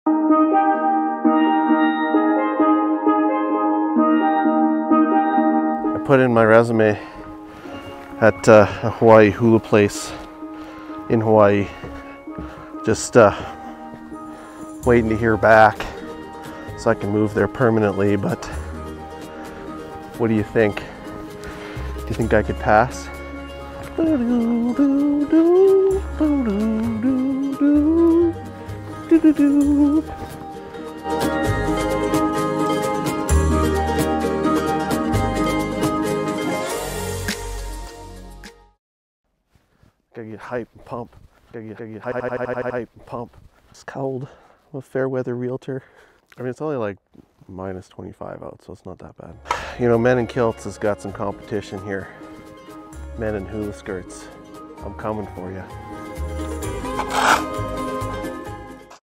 Listing a house in Hula Skirt……..in winter
realtorlife calgary yyc calgaryrealtor yycrealestate When the snow flies I always think of warmer weather. This listing day had me dreaming of a life outside of -25c weather. Knowing I couldn’t fly to Hawaii (I had to sell a house), I thought it best to bring Hawaii here. Well we ended up selling the house for a great price but not without some fun!